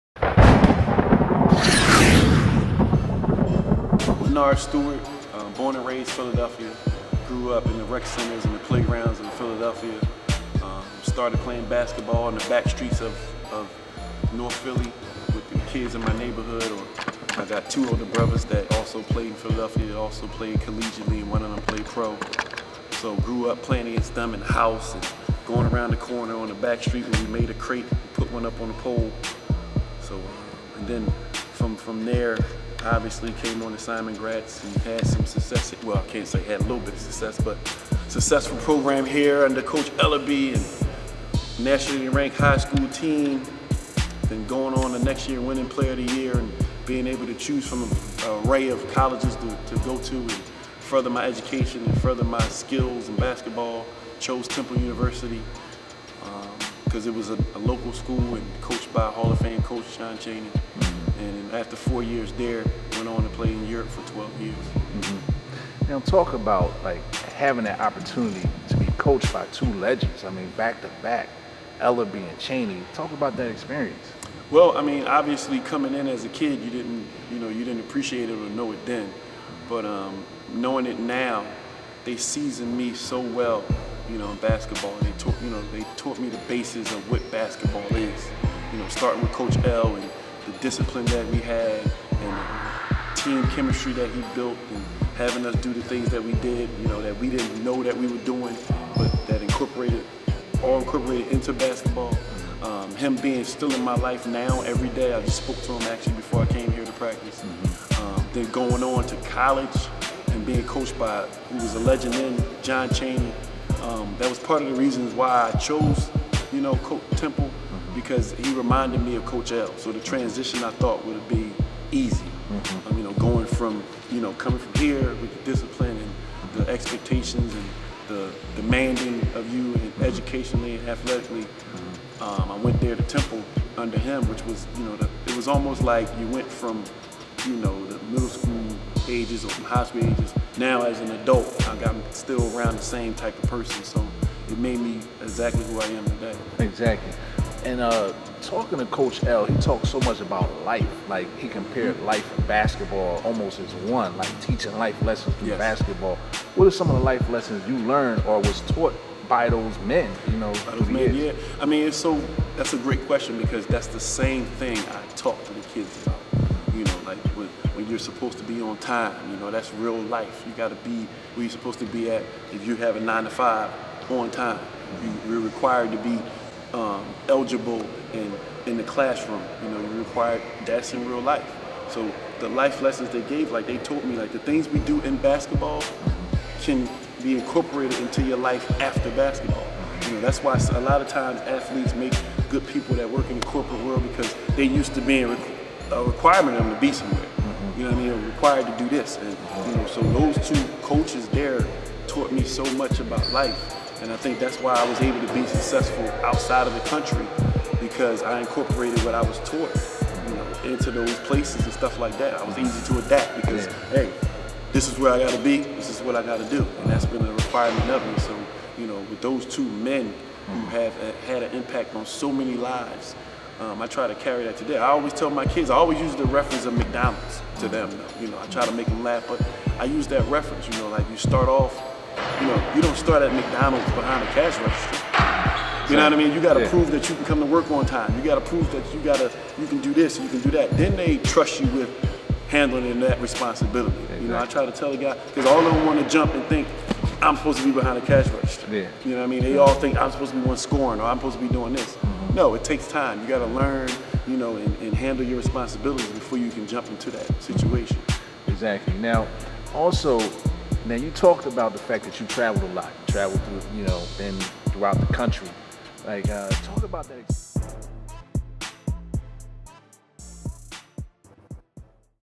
"Nardy" shares his experiences playing for 2 Philly Legends.
Simon Gratz Head Coach and former Philadelphia "Player of the Year", Lynard Stewart talks about the LIFE LESSONS he learned playing for 2 Philly Legendary coaches.